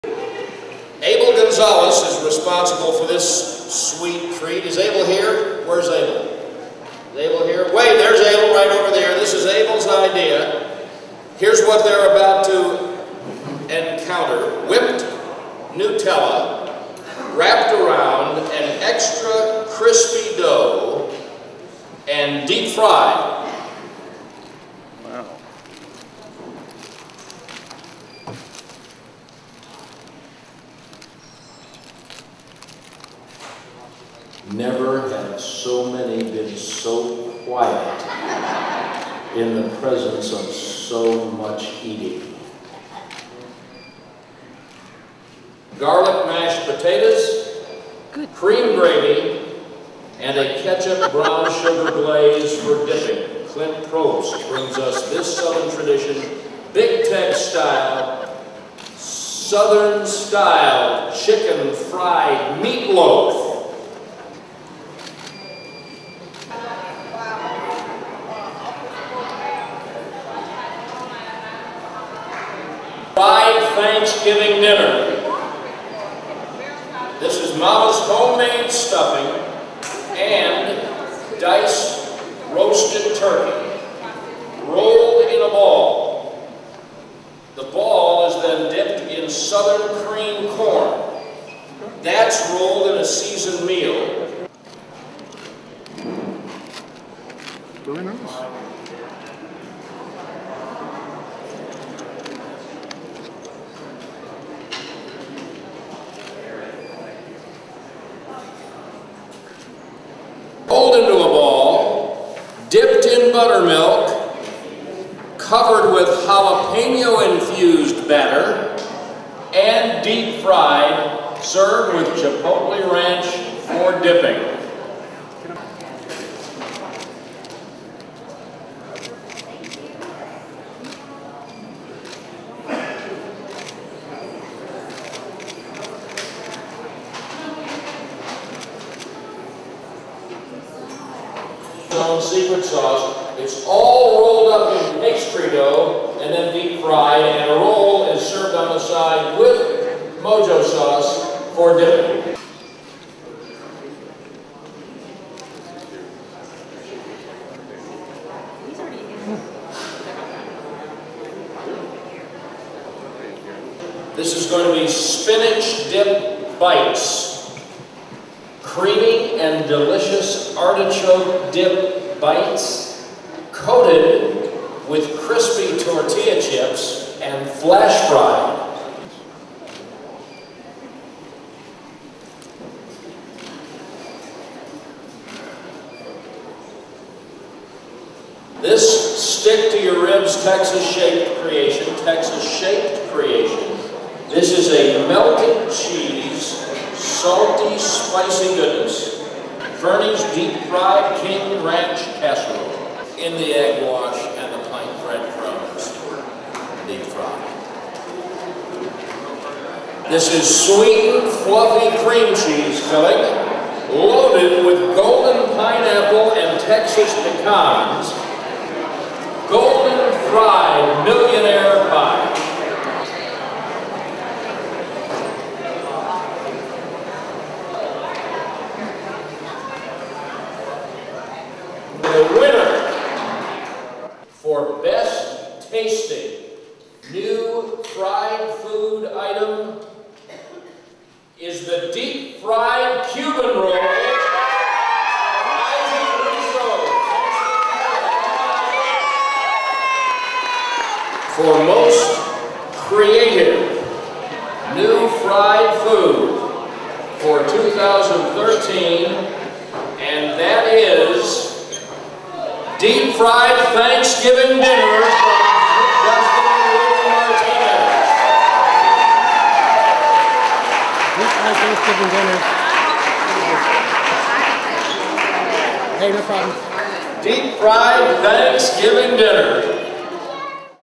2013 Big Tex Choice Awards
Judging the finalist of the 2013 Big Tex Choice Awards.